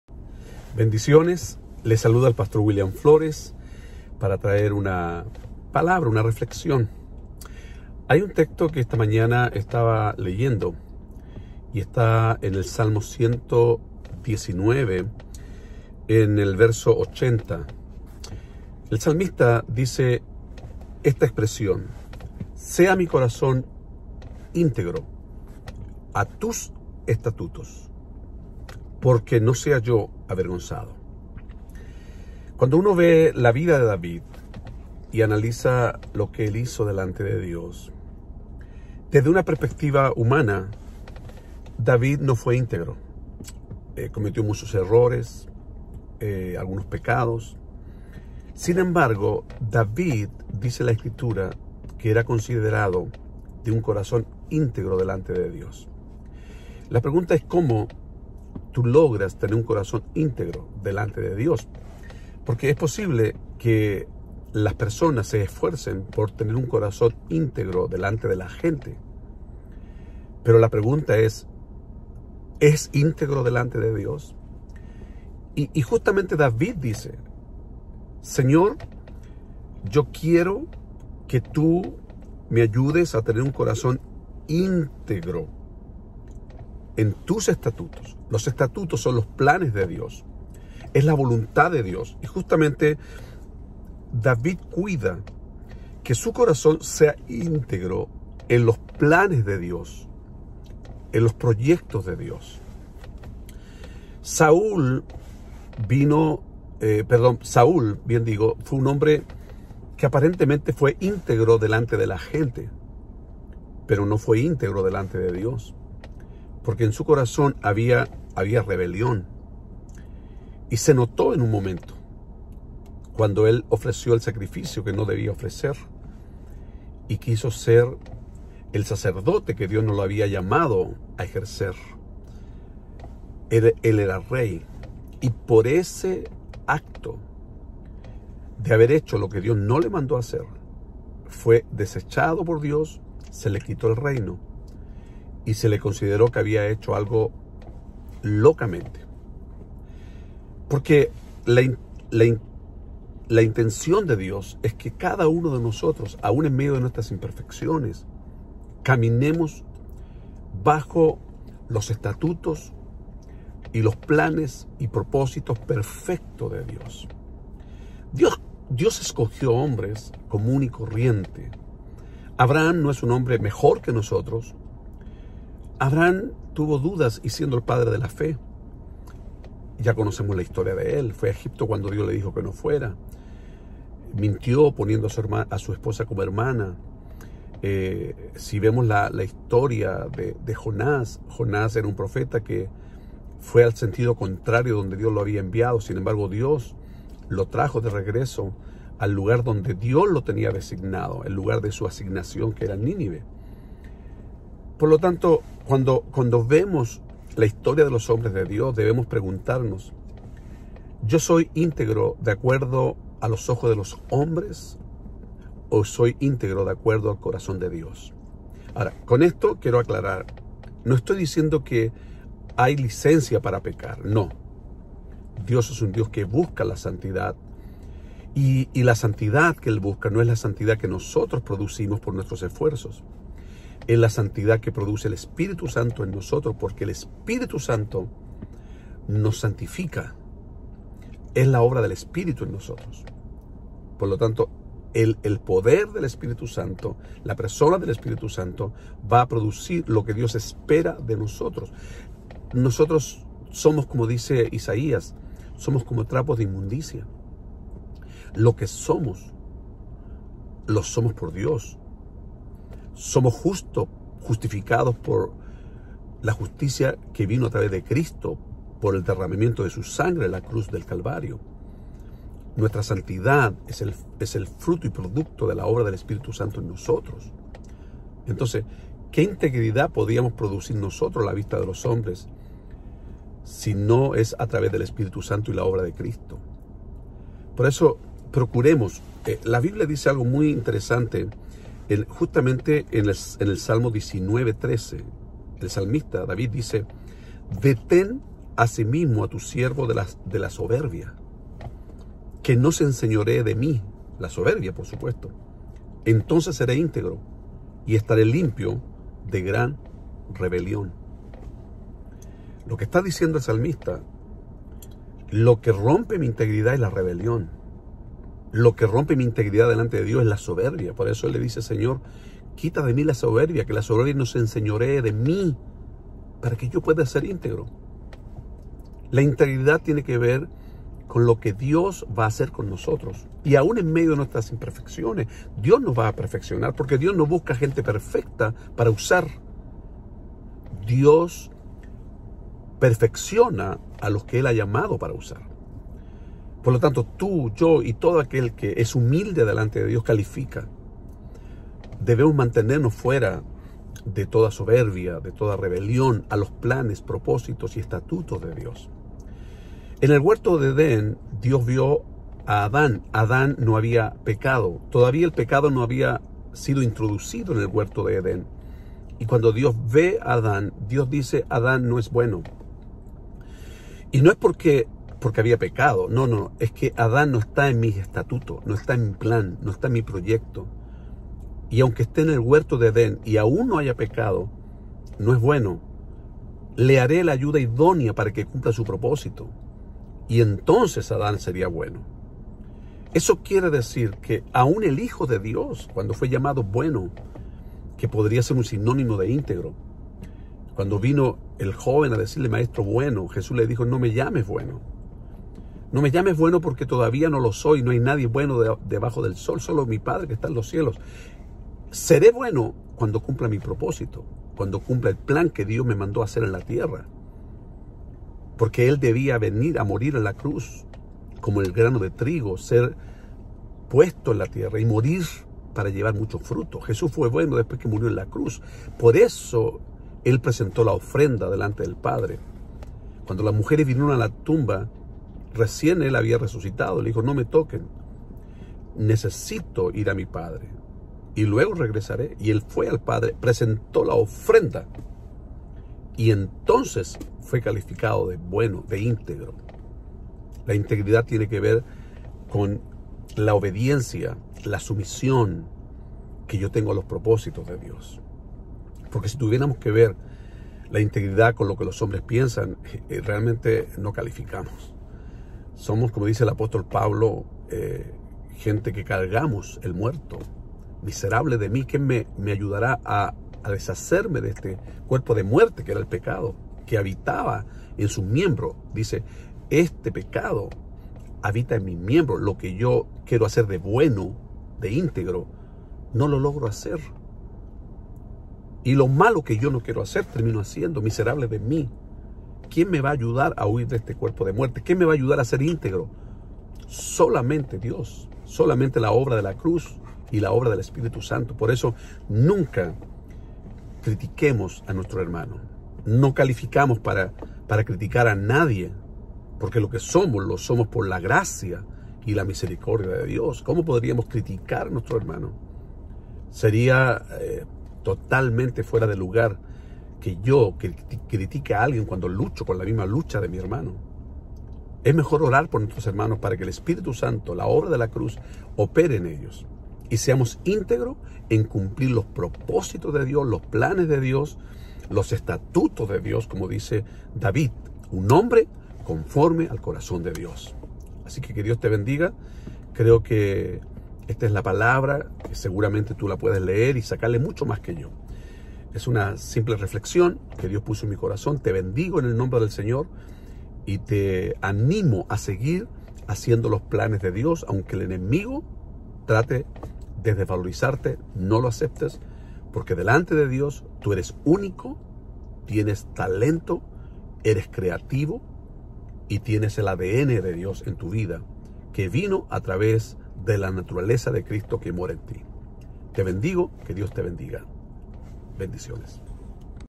Buscamos ser íntegros delante de los hombres, o delante de Dios?
Sea íntegro mi corazón en tus estatutos, para que no sea yo avergonzado. Salmos 119:80